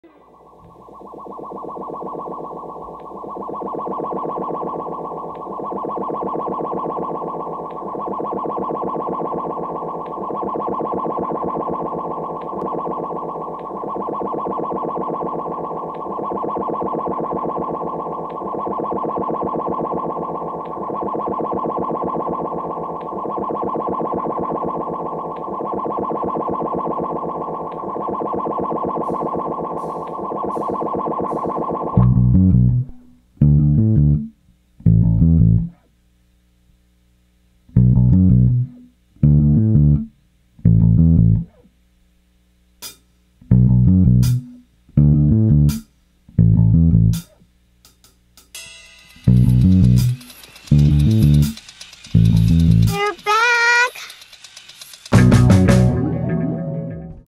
They're Back - Perpetual Groove returns to the Georgia Theatre
Perpetual Groove returns to the Georgia Theatre on 5/29/2015.  Tickets will go on sale 2/13/2015 at Noon EST.